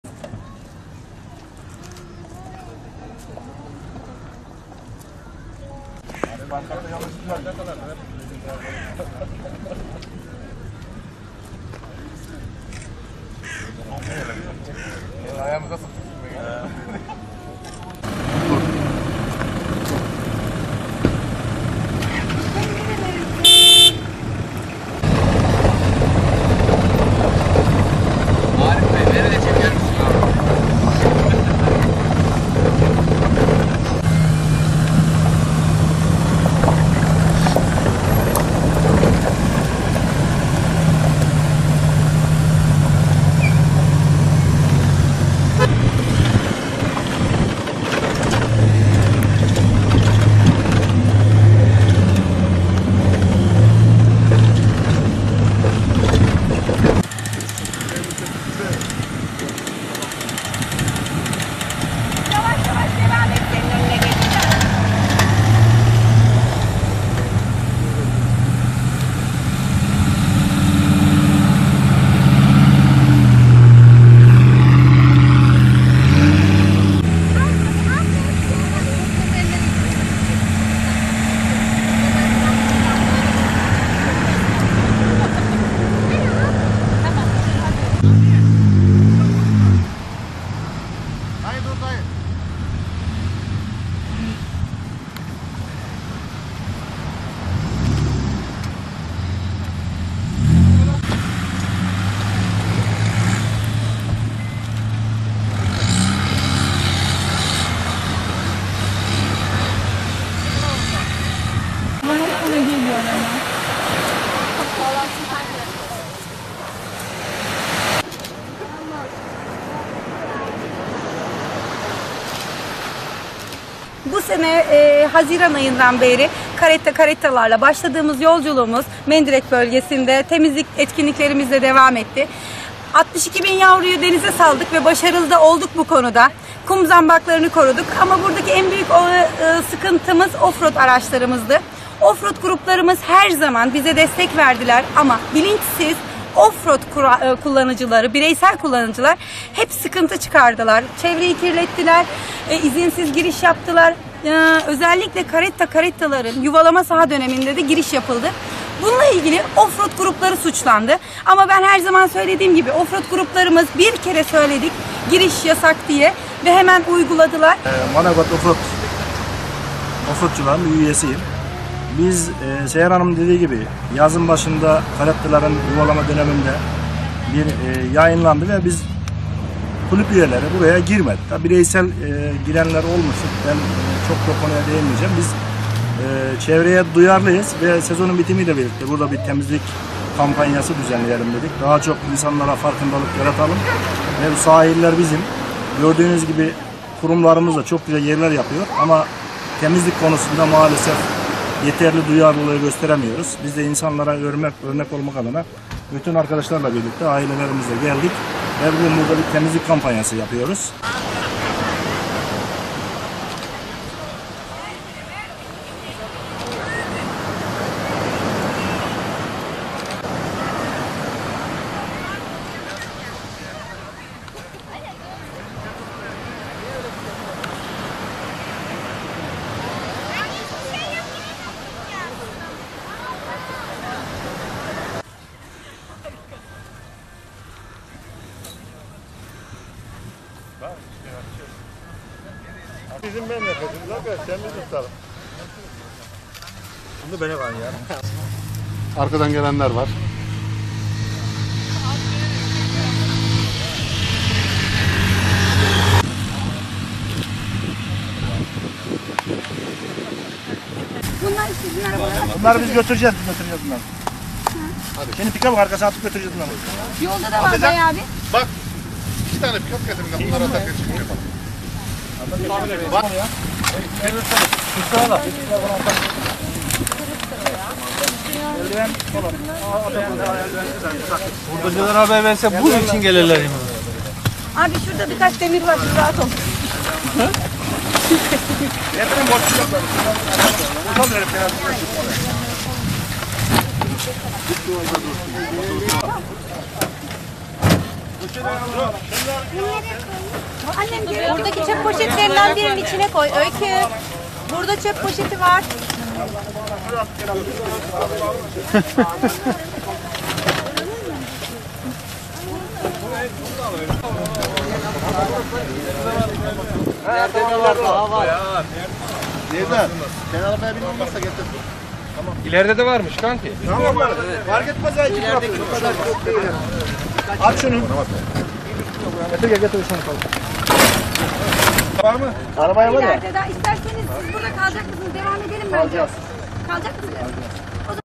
Manavgatlı Off-Road Tutkunlarından Sahilde Temizlik
Manavgat Off-Road Kulübü üyeleri aileleriyle birlikte sahillerde temizlik kampanyası gerçekleştirdi. Yapılan çalışma sonucunda mendirek bölgesinde onlarca çuval atık madde toplandı.